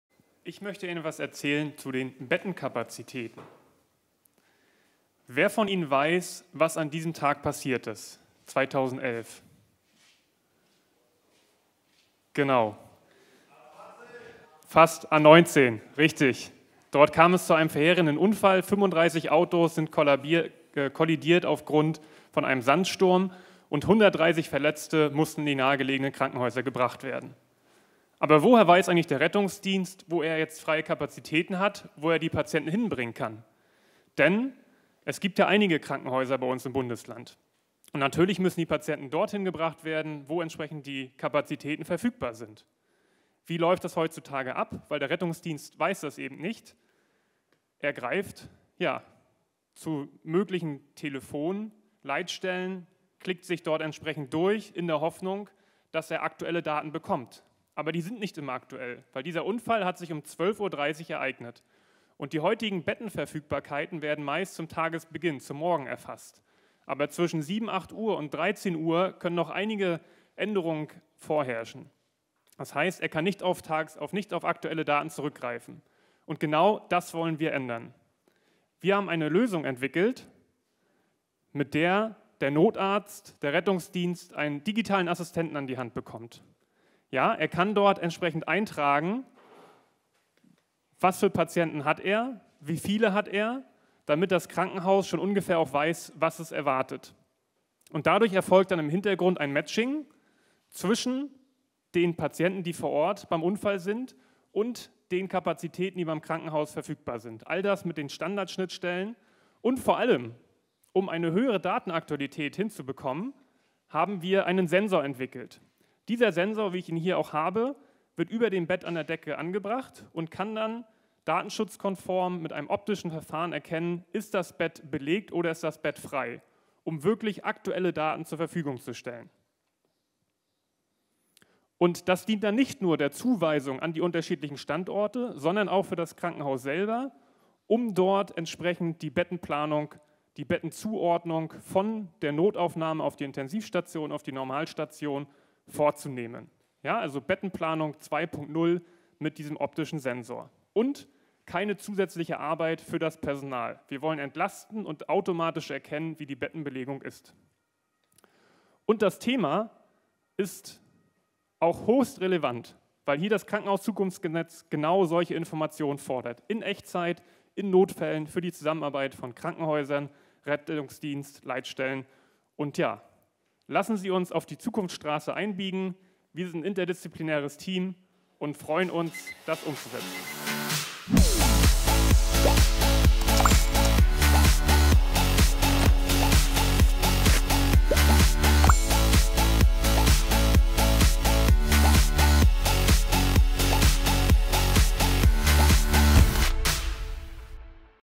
Healthcare Hackathon Greifswald 2021 - Pitch Platz 2 - Bettenmanagement (Team: Unter Druck)
Echtzeitabfrage von Bettenkapazitäten: Mit aktuell verfügbaren Systemen lassen sich automatisiert zwar tagesaktuelle Bettenkapazitäten digital zur Verfügung stellen. Benötigt man jedoch eine minutenaktuelle Verfügbarkeit, so lässt sich dies lediglich telefonisch in Erfahrung bringen. Im Notfall geht viel wertvolle Zeit verloren. Team "Unter Druck" schlägt eine Lösung dafür vor.